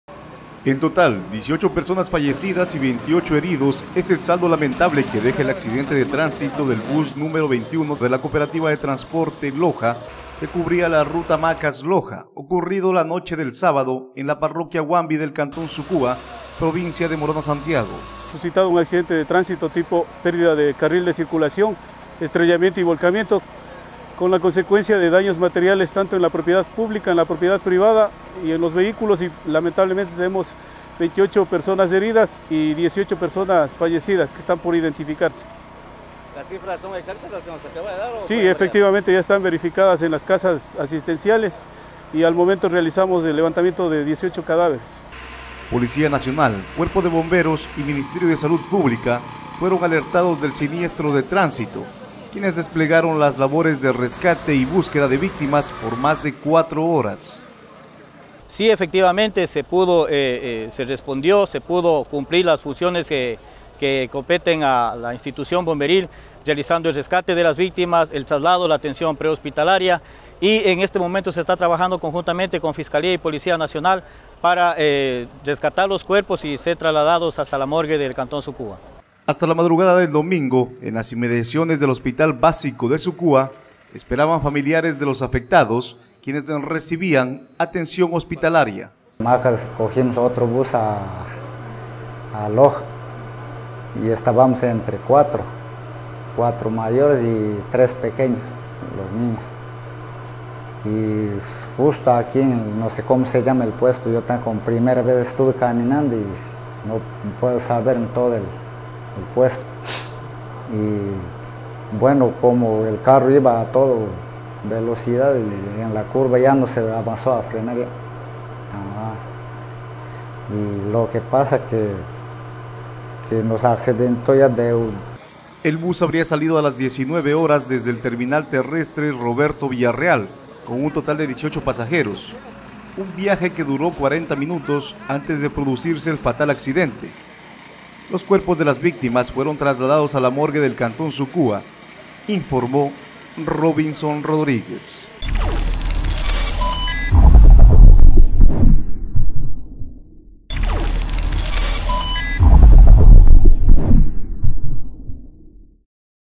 Trágico accidente de tránsito en Morona Santiago deja 18 fallecidos. Bus de la Cooperativa Loja
[EXPRESIONES] ATENCIÓN: MoronaSantiago, Efrain Santi, pasajero sobreviviente del accidente registrado en Huambi, señala que el bus se movilizaba con exceso de velocidad.

Policía Nacional, Cuerpo de Bomberos y Ministerio de Salud Pública fueron alertados del siniestro de tránsito, quienes desplegaron las labores de rescate y búsqueda de victimas por más de 4 horas.